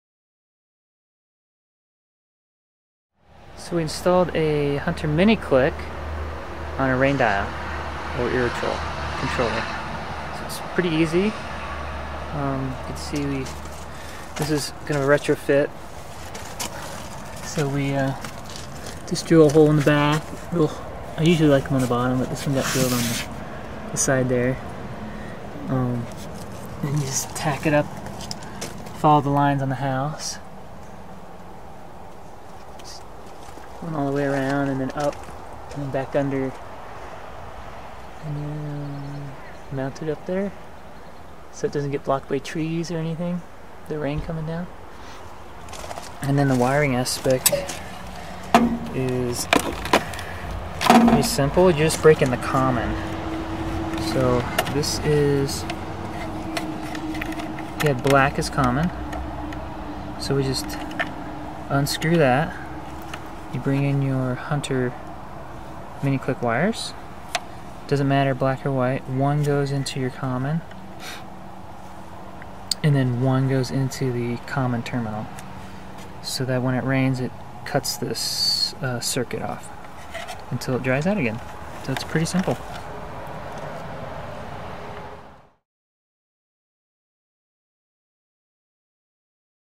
Wire A Rain Sensor To Any Controller - Raindial and Hunter Mini Clik
Easily install a wired Hunter Mini-Clik rain sensor to any brand of irrigation controller or timer.  This is a very easy process if you have some basic handyman skills.  The main idea is to interrupt the common wire with the rain sensor.  This way when there is enough rain to trigger the sensor then the common wire circuit is broken.  Kept this video short to show the wiring install.  We used staples for cables to tack down the wire.

0:49 Pointing out the wiring method inside the controller.  This happens to be an Irritrol Rain Dial irrigation controller.

Flagstaff, AZ